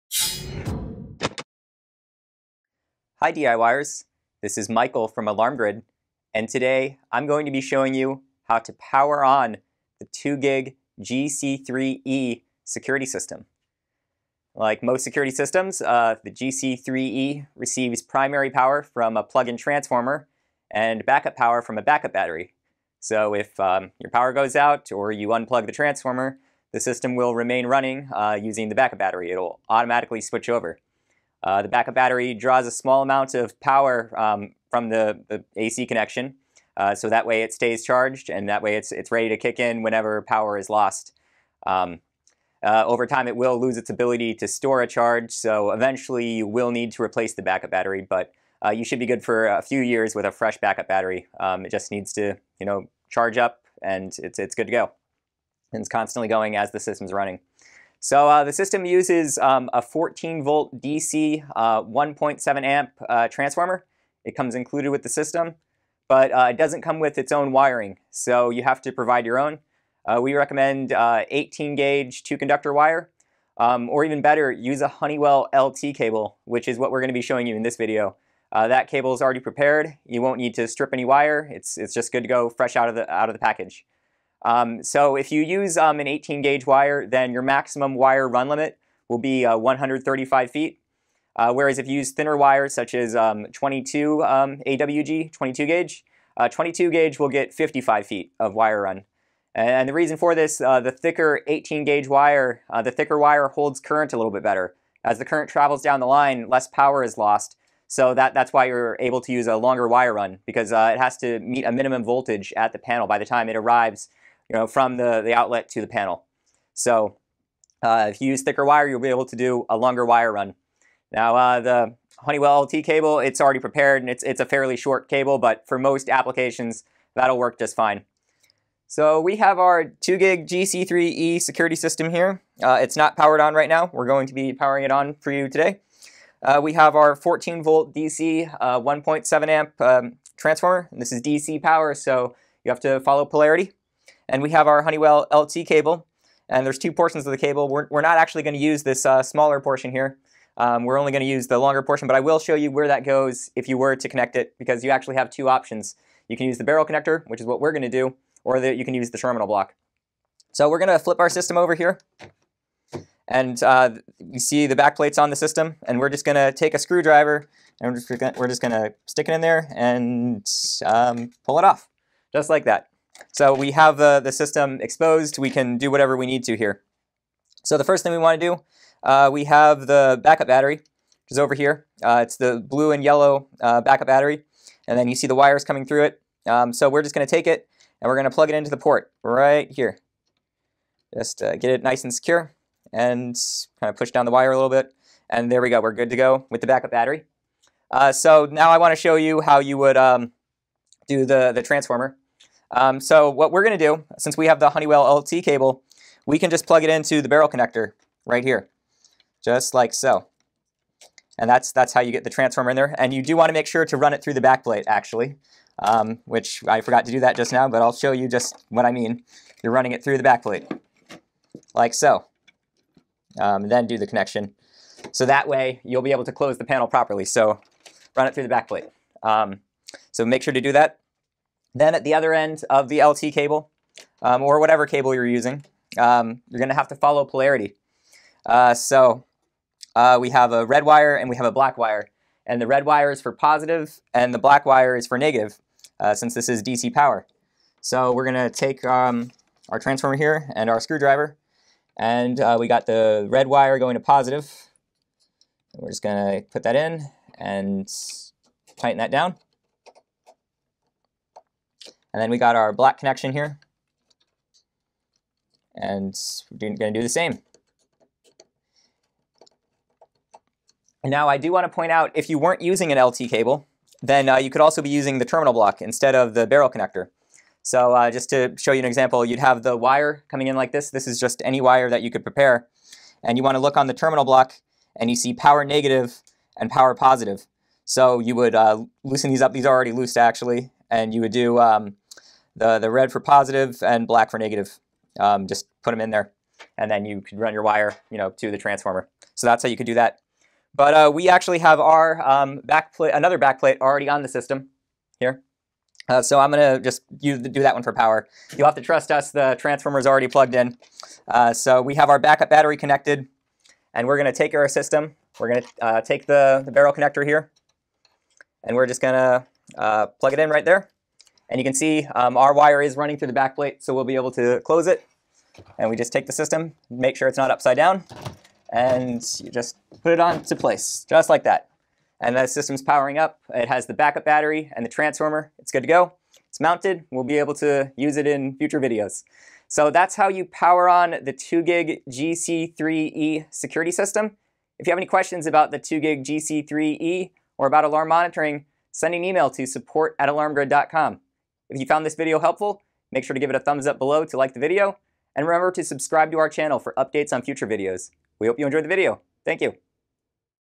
Powering On the 2GIG GC3e
In this video, Michael from Alarm Grid shows you how to power on the 2GIG GC3e. The 2GIG GC3e uses a plug-in transformer for primary power, and a battery for backup power. When performing the initial power on process, you should connect the backup battery first, followed by the transformer. The system uses a 14VDC, 1.7A transformer for power. The backup battery will need to be replaced every few years, as it will lose its ability to store a charge over time.

Alarm wiring is not included with the 2GIG GC3e Security System, so you must supply your own. Using thicker wire will allow for longer wire runs, which can be important in some installations. By going with 18-gauge wire, you can have a wire run of up to 135 feet. However, we recommend using a Honeywell LT-Cable if possible. The Honeywell LT-Cable is already pre-prepared, and it does not require any stripping or cutting.

There are two possible connections you can make at the panel for providing transformer power. If you are using conventional wire, then you must use the terminal block. But if you have the Honeywell LT-Cable, then you can connect at the barrel connector using the long portion of the Honeywell LT-Cable. Remember to run the wire through the back plate before making any connections so that you can properly close the panel. The back plate will click back onto the panel.

Since DC power is being used with the GC3e, you must observe polarity when wiring for power. Traditionally, the black wire is used for negative (-) power, and the red wire is used for positive (+) power. The included DC transformer will have the positive and negative terminals labeled. And if you are wiring at the terminal block, then you must observe polarity at those connections as well.